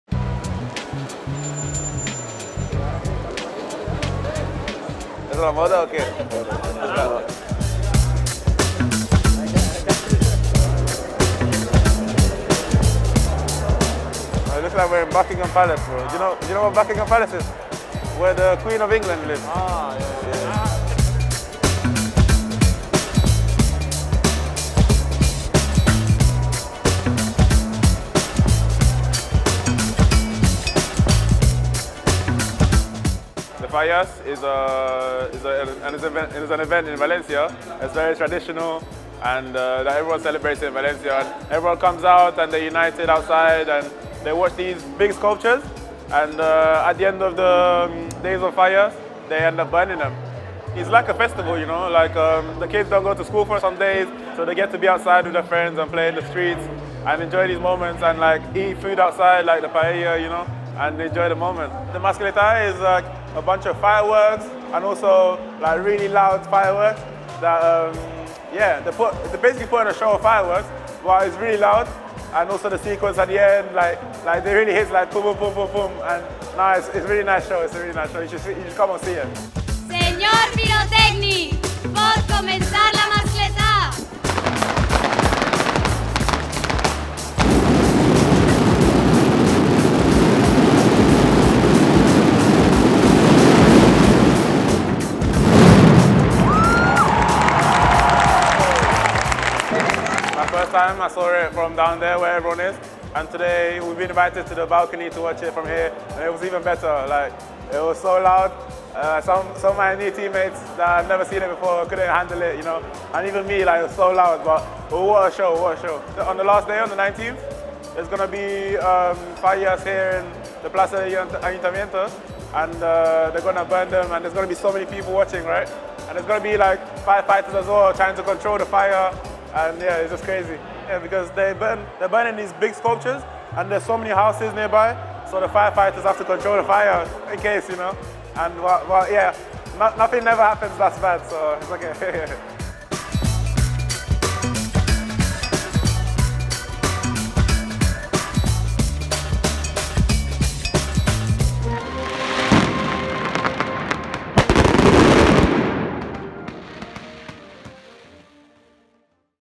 UNA MASCLETÀ CON YUNUS MUSAH
El pasado día 10 de marzo, el Valencia CF disfrutó de la mascletà fallera desde el balcón del Ayuntamiento. Desde ese lugar tan privilegiado para los valencianos, Yunus Musah vivió de manera diferente uno de los actos más significativos de las Fallas. Esta fiesta, que acaba de entrar en sus días grandes, es la más importante de la ciudad y Yunus la ha explicado para el público internacional que todavía no la conoce.

On March 10th, Valencia CF enjoyed the "mascletà" fireworks from the Valencia city council balcony. A special location for Valencians. Yunus Musah experienced one of the most significant events of the Fallas festivities in a different manner, as we have just entered into the height of the festivities.
The US midfielder  took the opportunity to describe the festivities to our international audience.